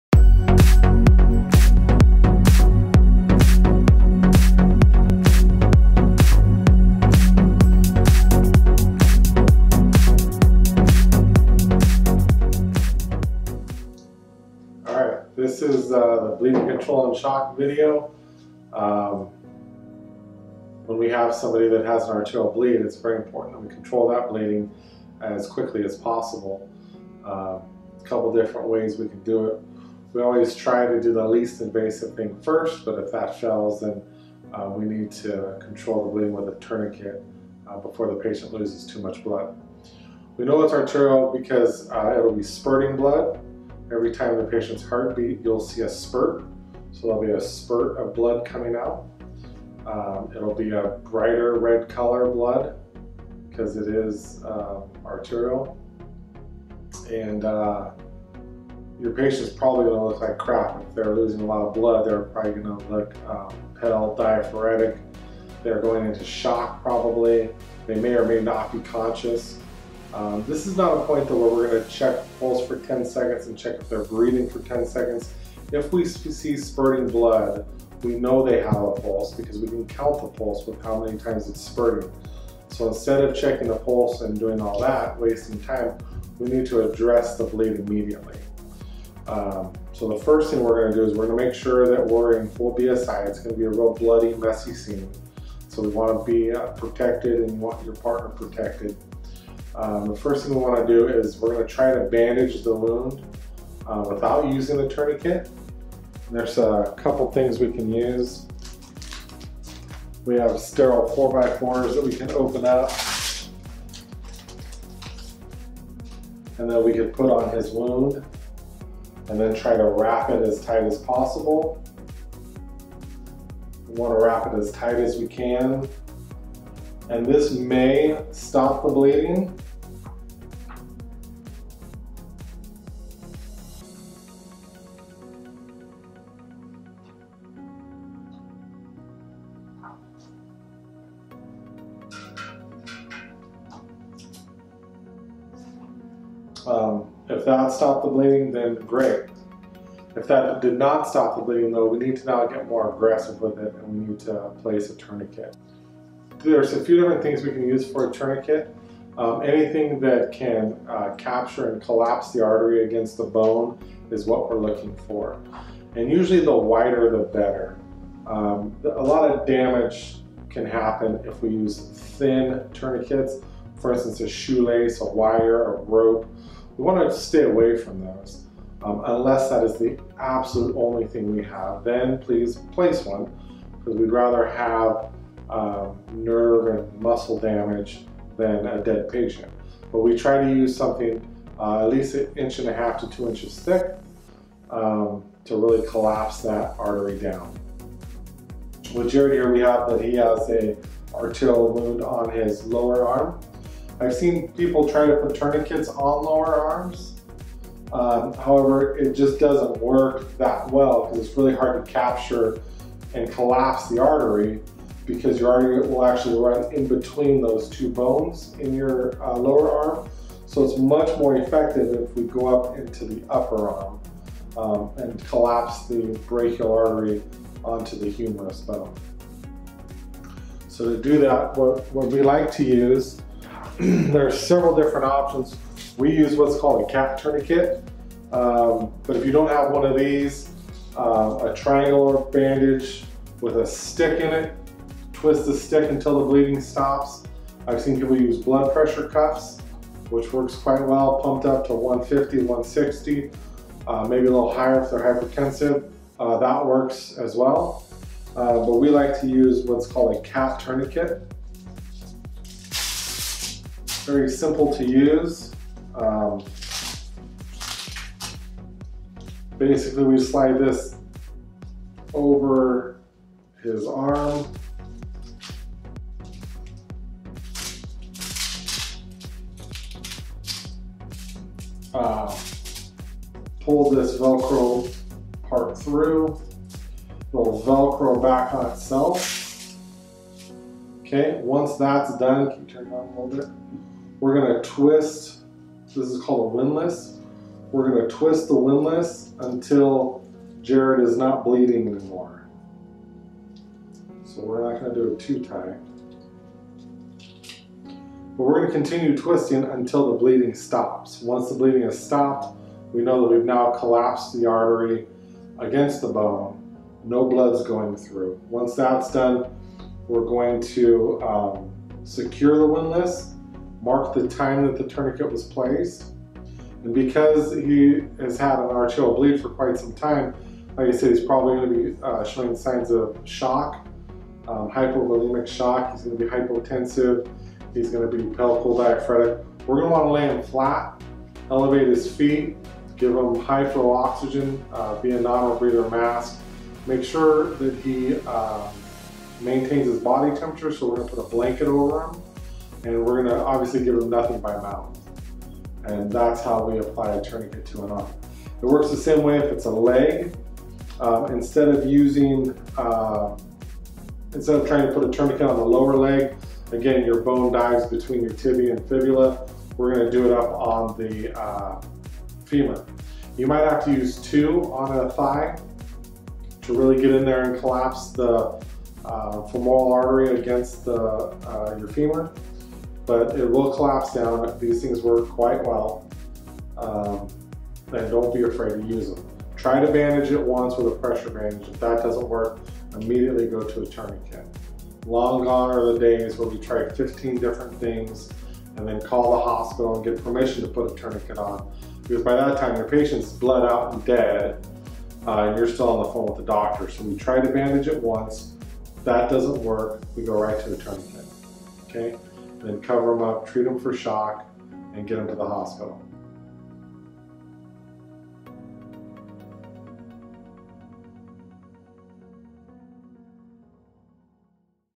EFT Bleeding Control and Shock
EMT & Fire Training provides an EMS skill video on Bleeding Control and Shock Management